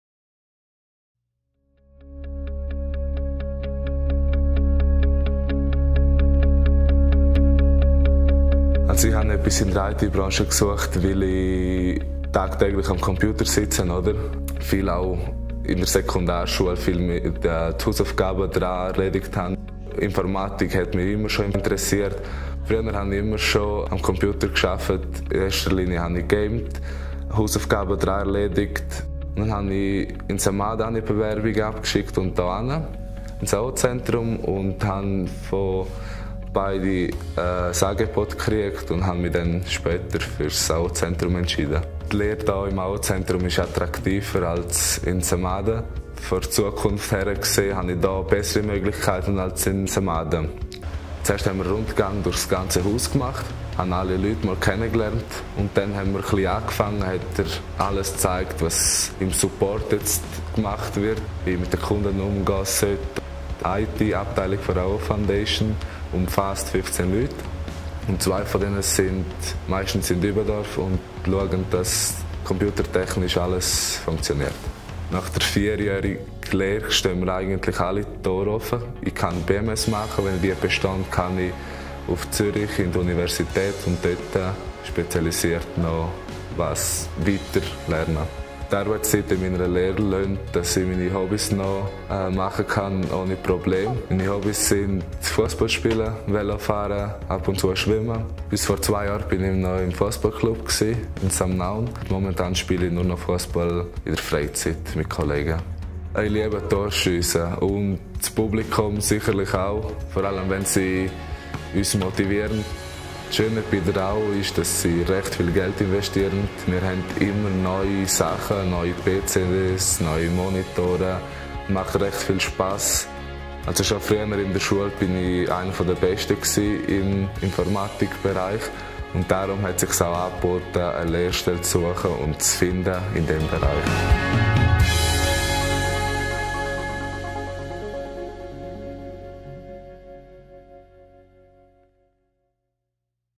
Die Informatiklehre - 4 Jahre Spannung und Abwechslung
Das 1. Lehrjahr ist dem Kundensupport gewidmet, wo dem Lehrling die Grundlagen der Informatik vermittelt werden, wie zum Beispiel Hardware und Software. Im 2. Lehrjahr werden die diversen IT-Gruppen durchlaufen. Als erste die Infrastrukturgruppe mit den Themen Netzwerke, Speichersysteme, Server-Virtualisierung und Firewall. Als zweite die Server-Applikationsgruppe mit den Themen Benutzerverwaltung, Serversoftware-Installationen und Monitoring. Die dritte Gruppe beschäftigt sich mit der Softwareentwicklung und dem Testen von Software. Ebenfalls lernt der Informatiklehrling das Beschaffen von IT-Material kennen. Im 3. Lehrjahr arbeitet der Lehrling in denjenigen Abteilungen der AO Foundation, die nicht zur IT gehören. In diesen Abteilungen vertieft er seine erworbenen Fähigkeiten und setzt sein Fachwissen in die Praxis um. Im 4. Lehrjahr schliesslich realisiert der Lehrling selbstständig ein herausforderndes IT-Projekt, das dem neusten Stand der Technik entspricht. Dadurch wird der Lehrling ideal auf die Lehrabschlussprüfung vorbereitet.

Die Lehre als Informatiker in der AO Foundation ist sehr empfehlenswert, weil diese Ausbildung extrem spannend und vielfältig ist.

Mehr über die Informatiklehre in der AO Foundation erfährst du